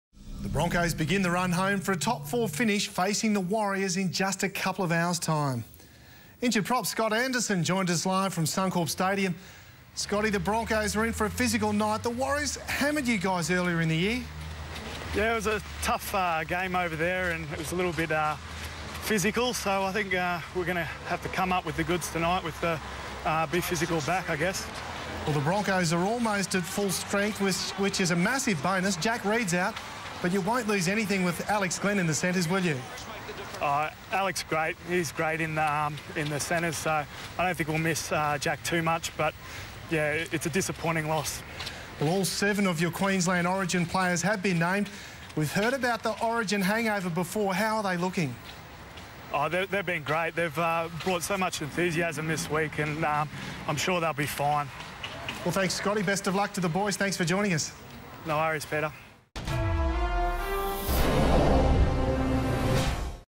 Broncos warriors
The Broncos begin the run home for a top four finish facing the warriors in a couple of hours' time.